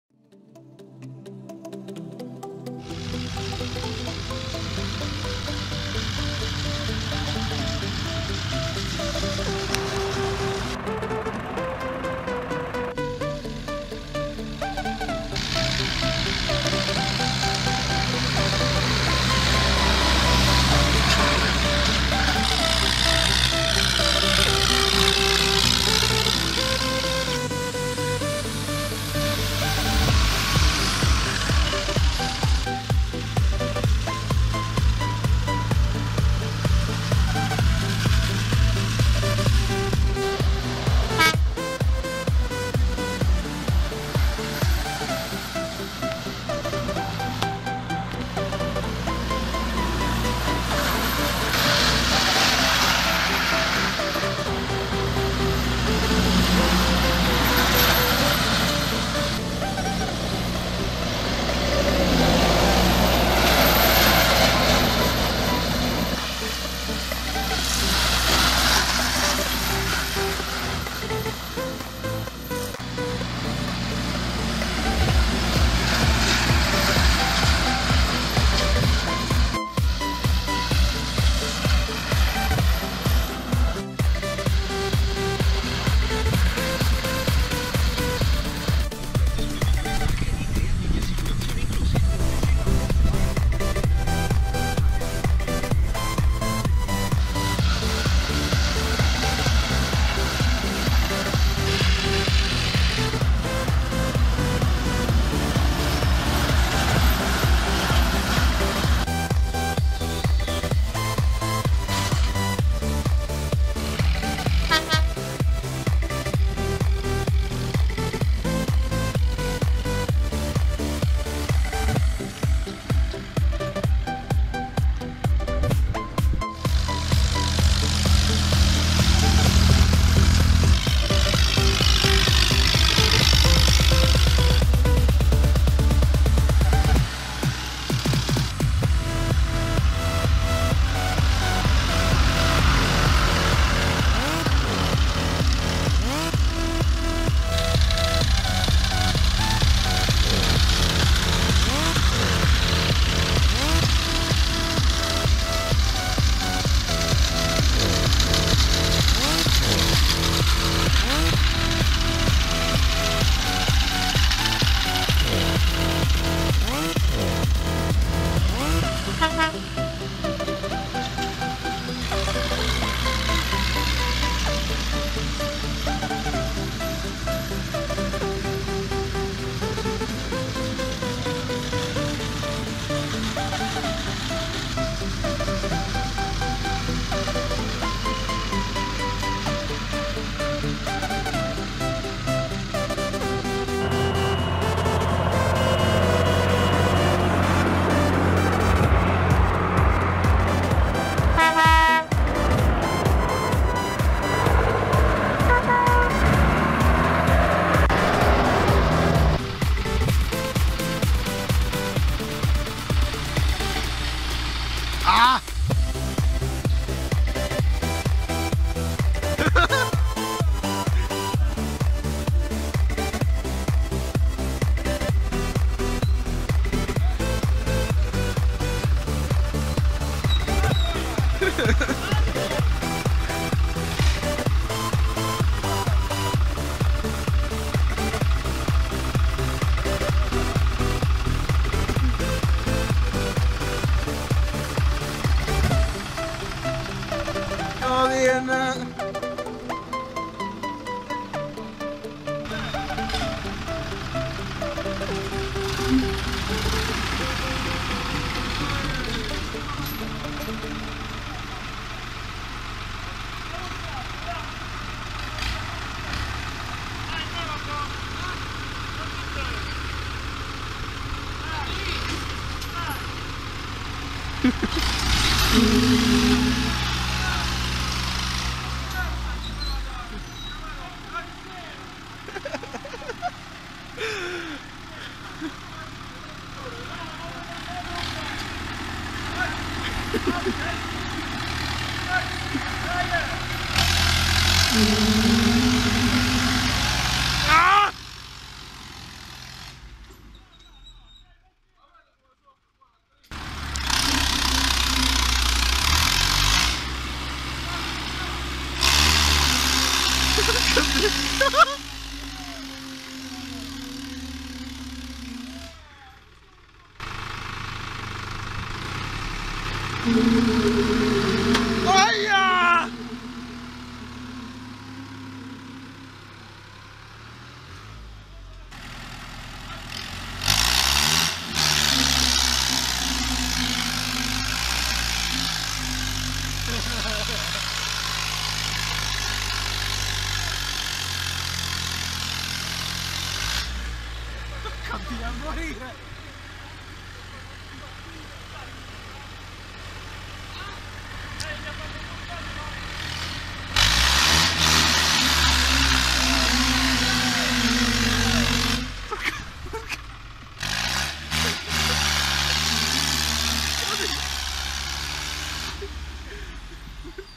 [AZIENDA PORTELLA] Sfalcio erba primo e secondo taglio e gara di tiro finale
Ci risiamo con la fienagione , con lo sfalcio presso l'azienda agricola PORTELLA con gara di tiro finale tra Fiat 640dt e New holland tn 60
i mezzi impegnati sono:
john deere 6120 con barra a dischi Kuhn
Agrifull 90 deluxe , Fiat 640dt , Fiat 500dt , New Holland tn 60 tutti e quattro con barre gaspardo fbr 205
Arrivederci al prossimo video con la ranghinatura e la pressatura